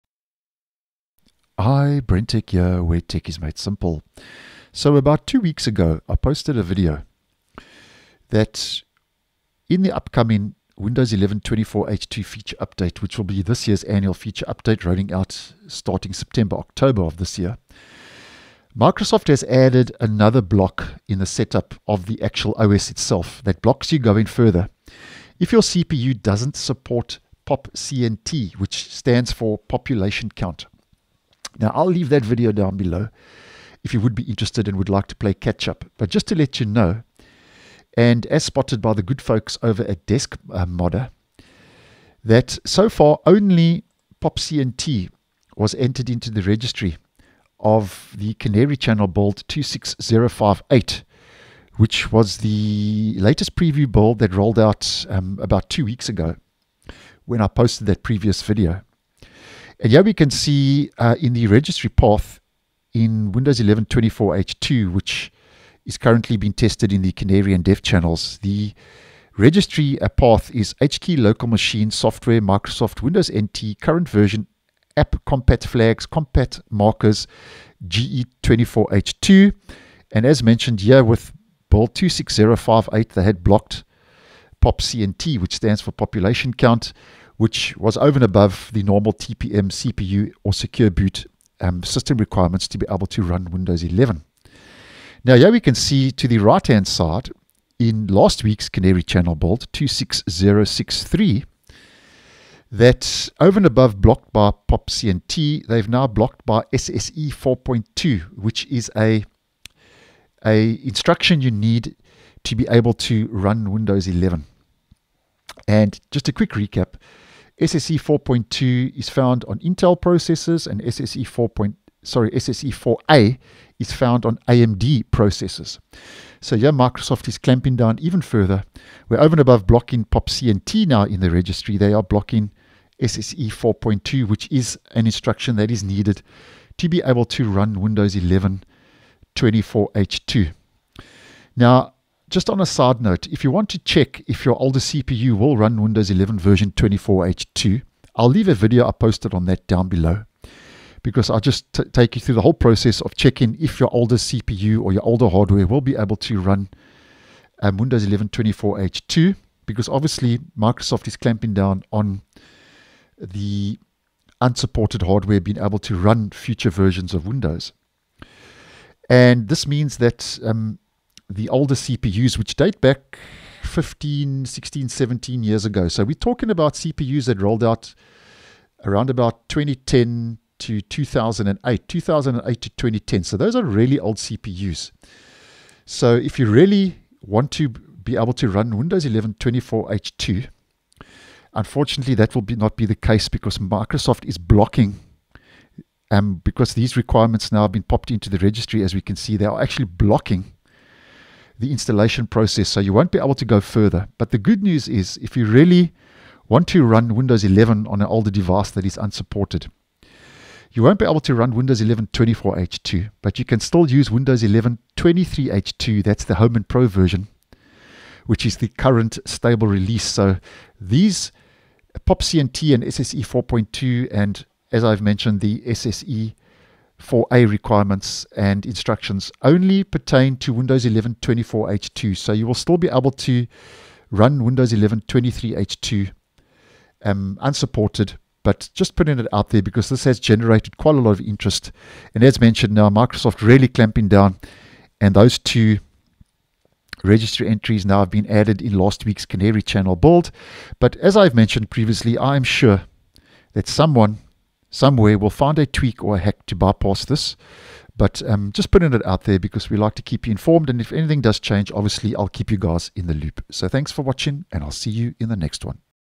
Microsoft Clamps Down Further to Prevent Windows 11 24H2 Running on Unsupported Hardware
As spotted by Deskmodder Windows 11 24H2 Registry now also contains BlockedBySSE4.2 to exclude old CPUs.